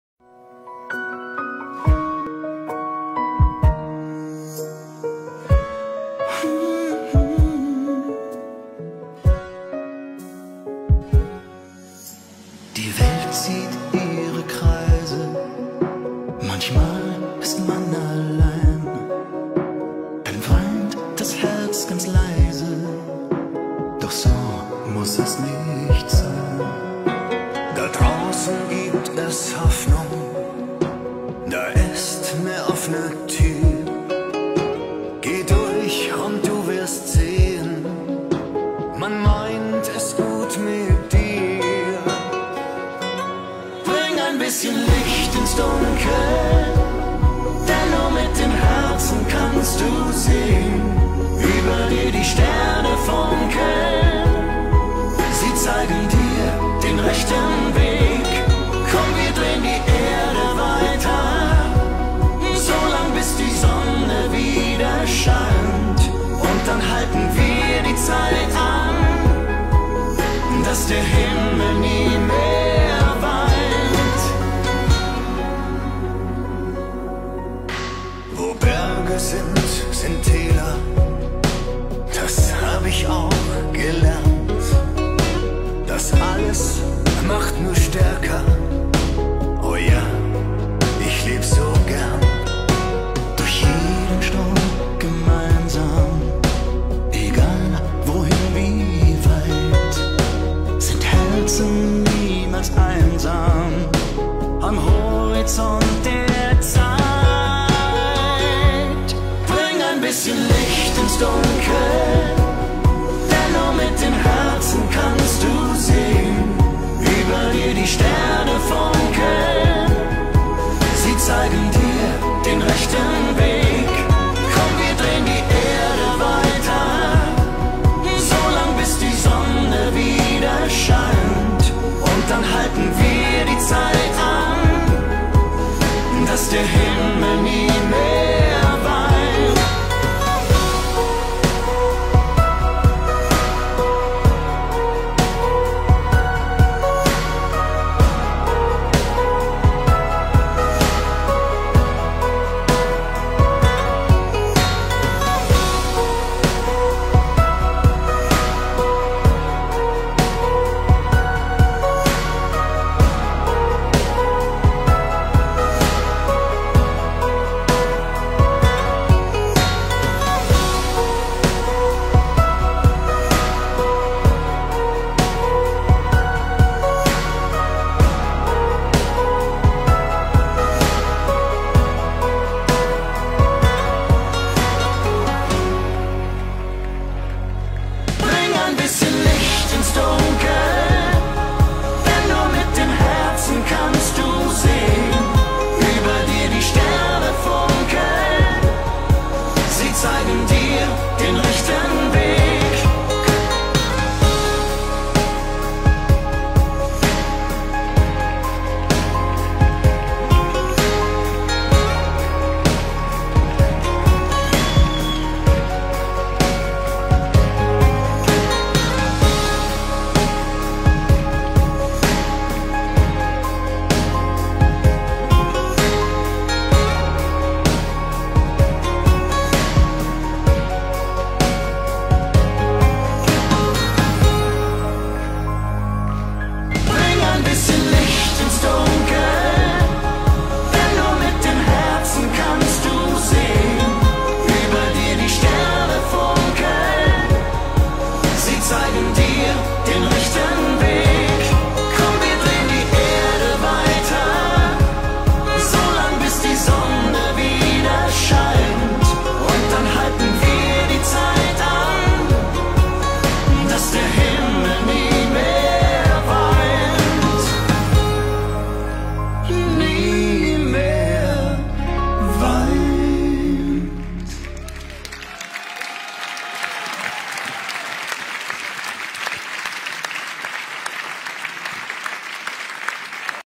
Thomas Anders & Florian Silbereisen. Gala "Licht ins Dunkel". Austria. 18.11.2023.
Actuación de nuestro fantástico dueto, Thomas y Florian, en la gala benéfica "Licht ins Dunkel" en Linz, Austria. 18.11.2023. 😊